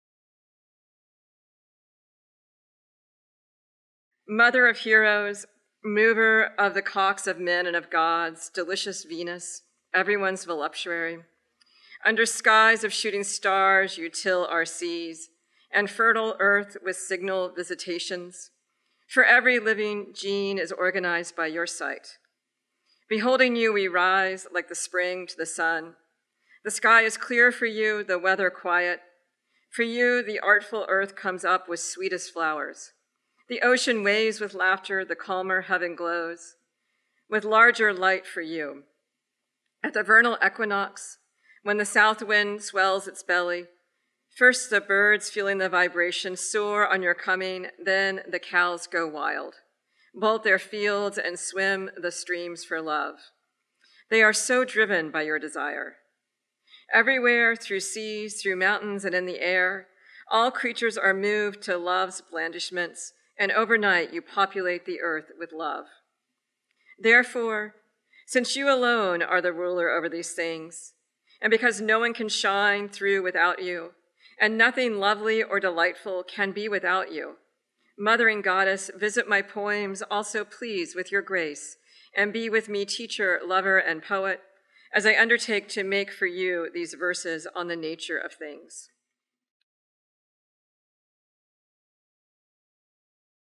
The Poetry Center — Stephen Rodefer: a Memorial Tribute
Juliana Spahr reads “After Lucretius” from Stephen Rodefer’s One Or Two Love Poems From The White World (The Pick Pocket Series No. 2, 1976), November 21, 2015, at The Poetry Center’s event, Stephen Rodefer, A Memorial Tribute, at Berkeley Arts Festival. The full program includes some twenty Bay Area poets reading (mostly) from Rodefer’s work, and offering tributes, and incorporates a portion of an historic video from The Poetry Center’s May 8, 1975 reading by Stephen Rodefer.